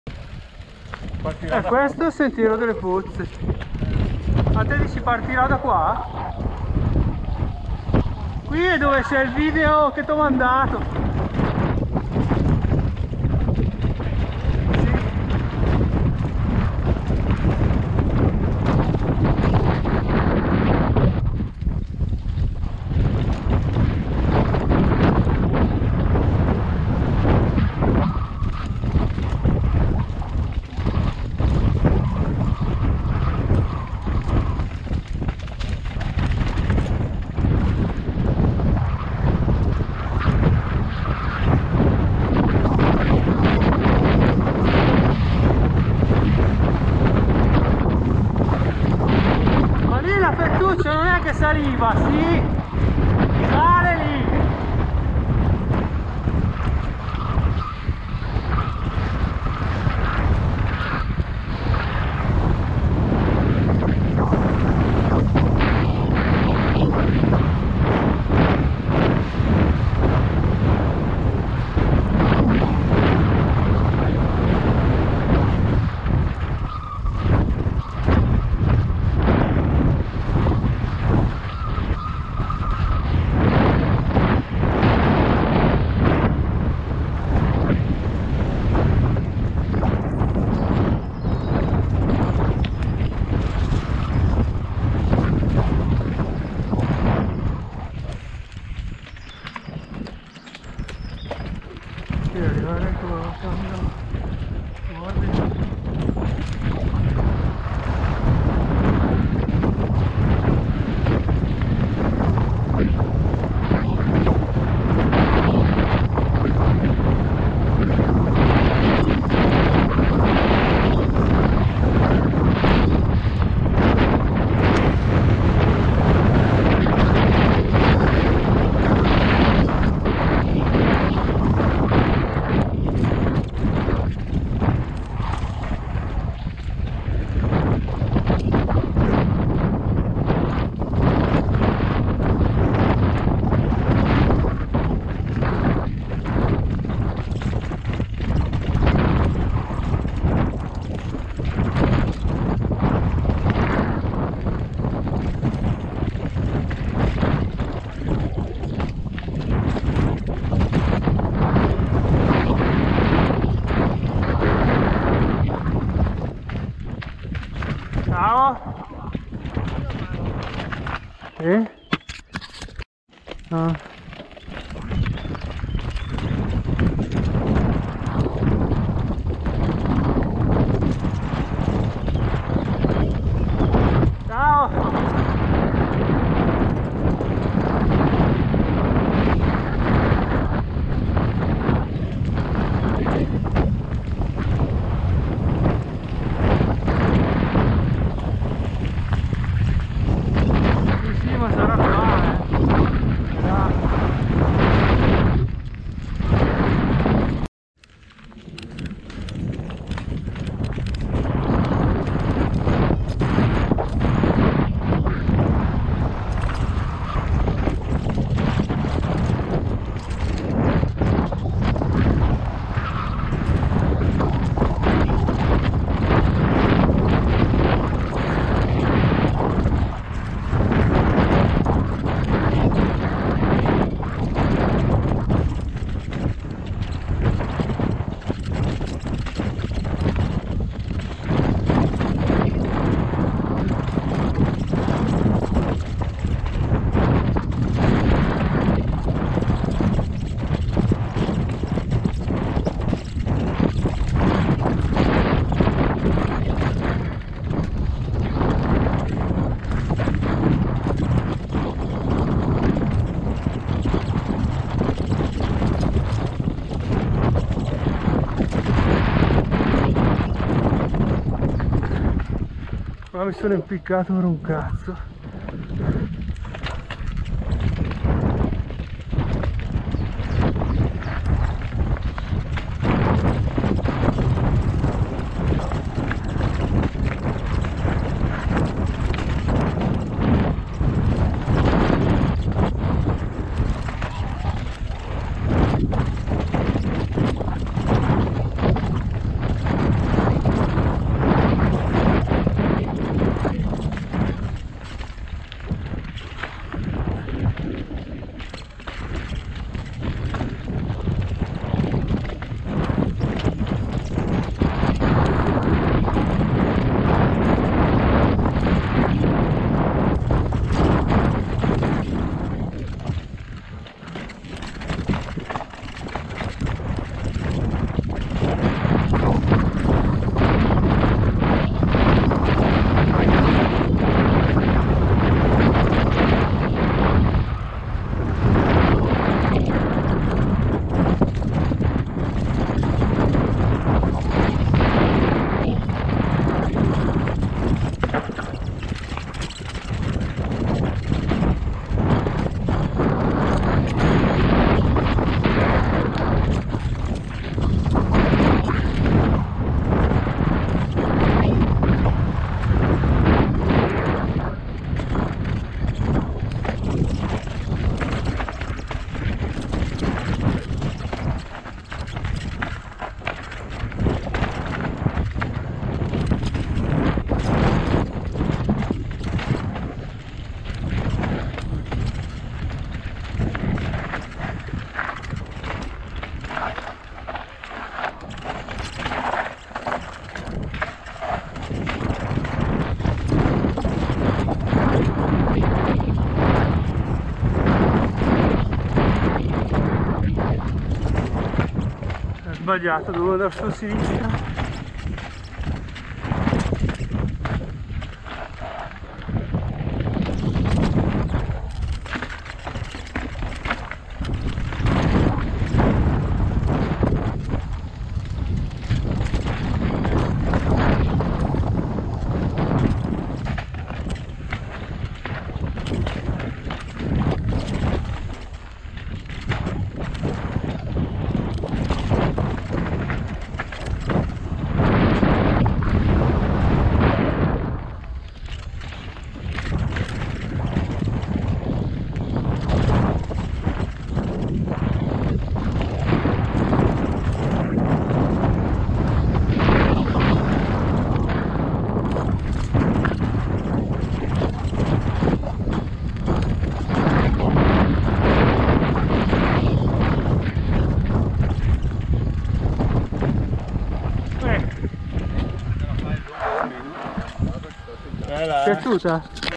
Anteprima Ps3 Gran Prix Enduro Brescia
Parte col sentiero delle pozze e prosegue facendo tutta la musa.